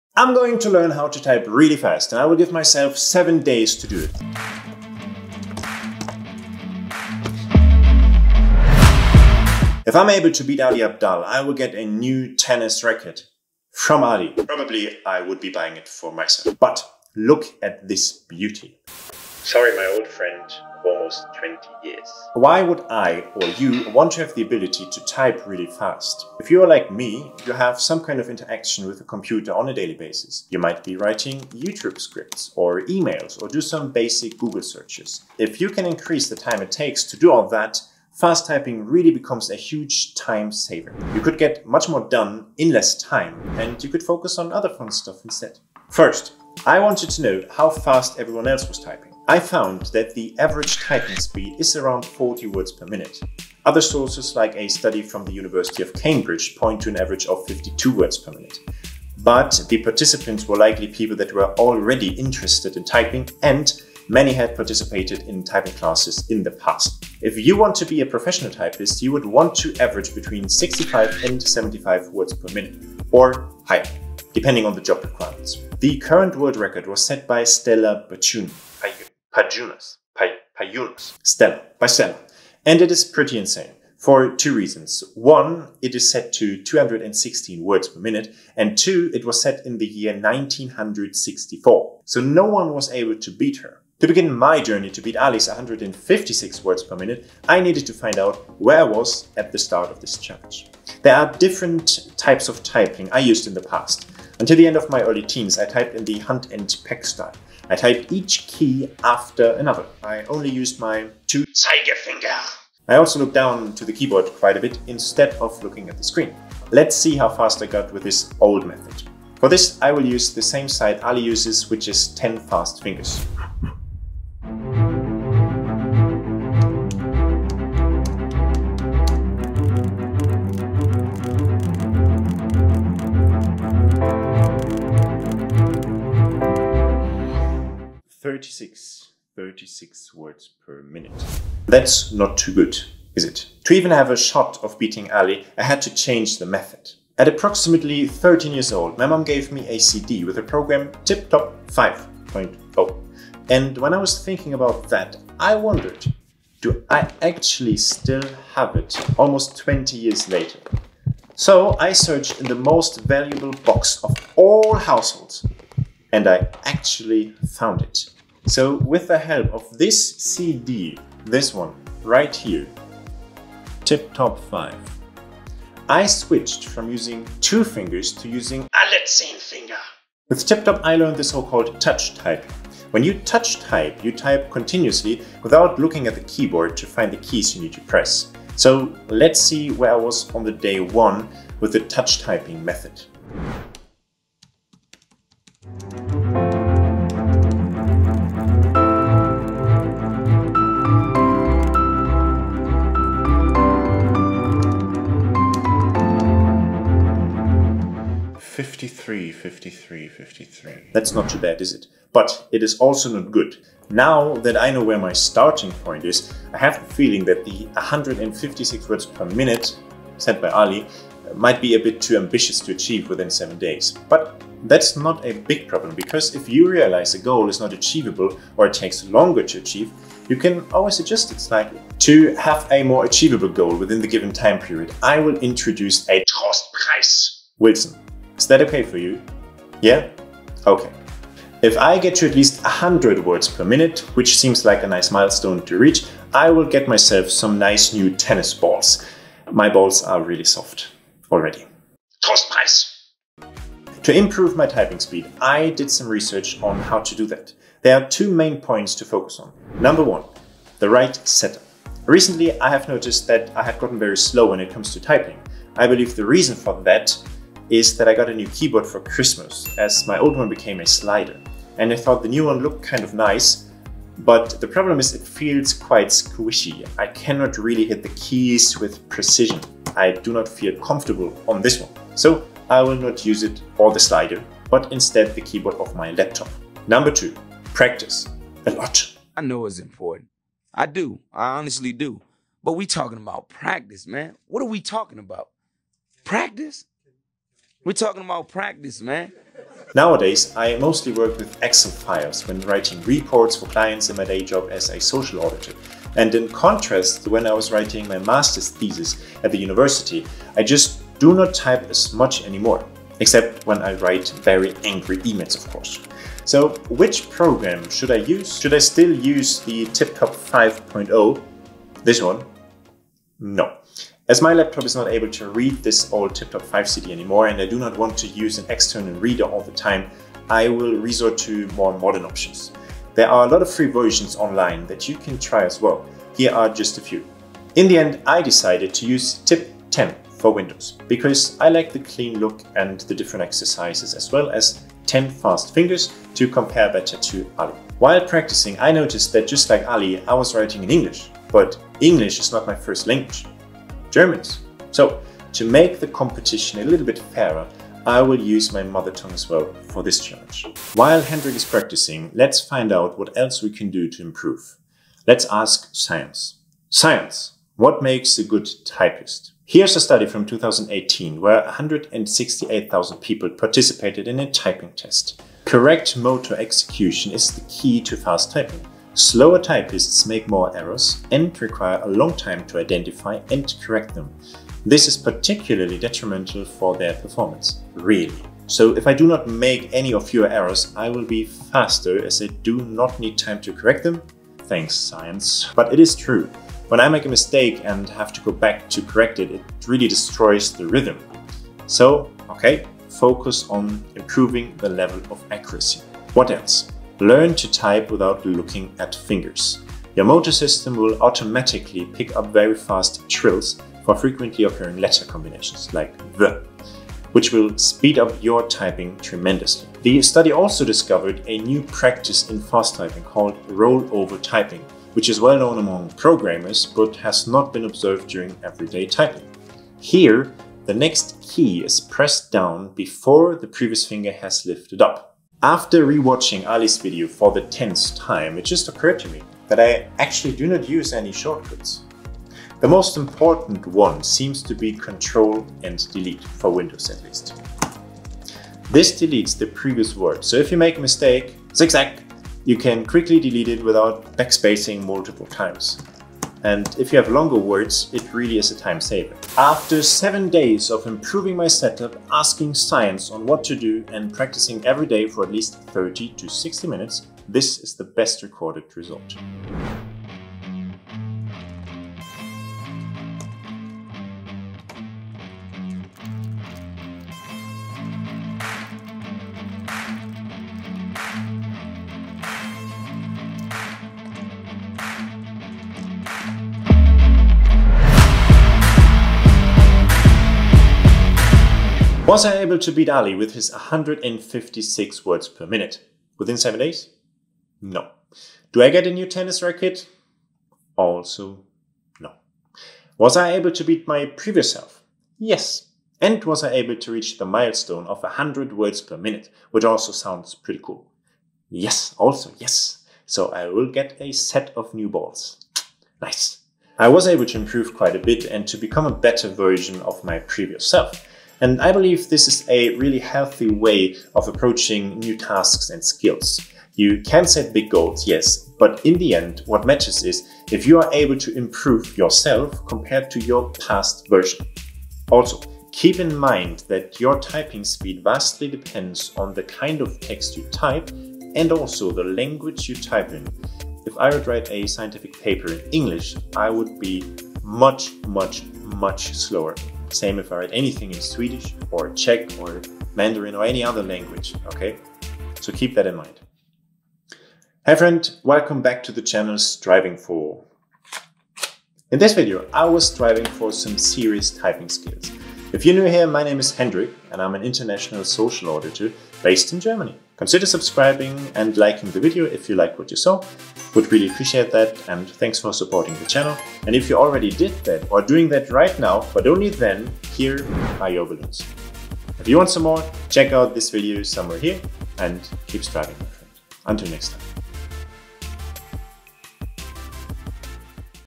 How I Learned To Type Really Fast - In 7 Days
I practiced Touch Typing for 7 days.

Goal 1: Beating Ali Abdaal who types around 156 words per minute
Goal 2: Increase my personal speed of around 53 words per minute (WPM) to over 100 words per minute.

The current typing world record was set by Stella Pajunas and it is pretty insane, for two reasons. One, it is set to 216 words per minute, and two it was set in the year 1946.

WHY would I or you want to have the ability type really fast?
If you are like me, you have some kind of interaction with the computer on a daily basis. You might be writing YouTube scripts, emails or do some basic google searches. If you can increase the time it takes to do all that, fast typing really becomes a huge time saver. You would get more done in less time and you could focus on other fun stuff instead.